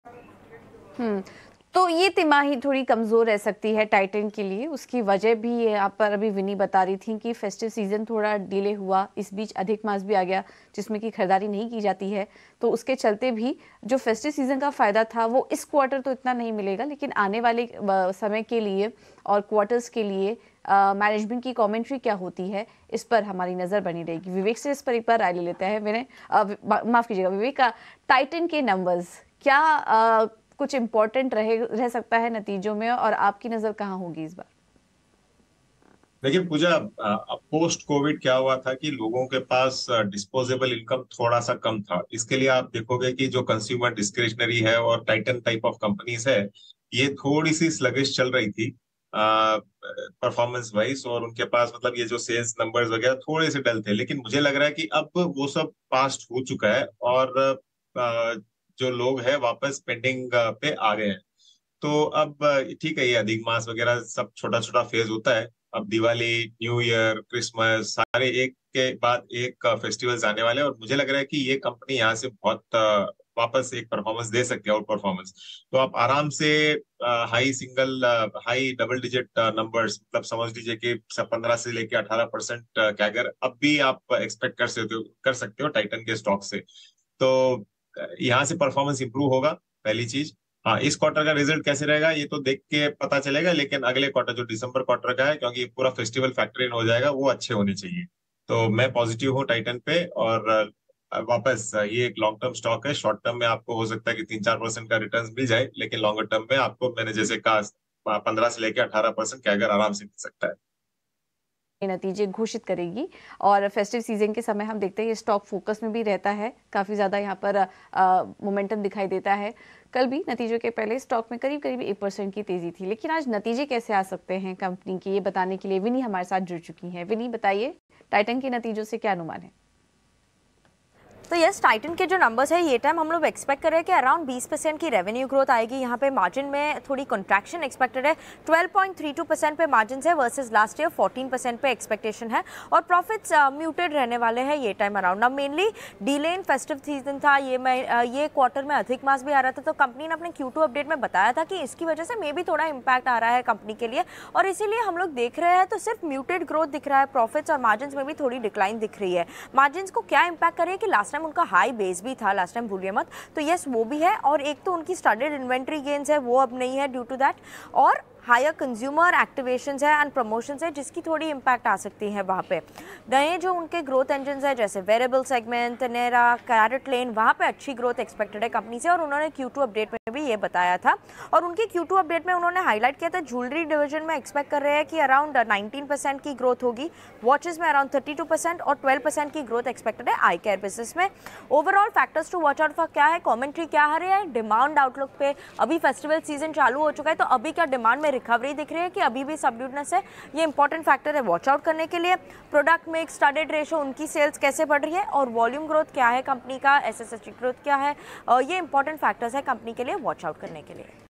Titan के Q2 नतीजों पर क्या है अनुमान ? Share में Long Term में मुनाफे पर Expert ने क्या कहा ? | ETNS
Titan Share में जानें निवेश को लेकर क्या है Experts की राय. जानें किन levels पर करनी चाहिए आपको खरीदारी और क्या है Target Price और जानिए किस Strategy के साथ इस Stock में पैसा कमाया जा सकता है. देखें वीडियो.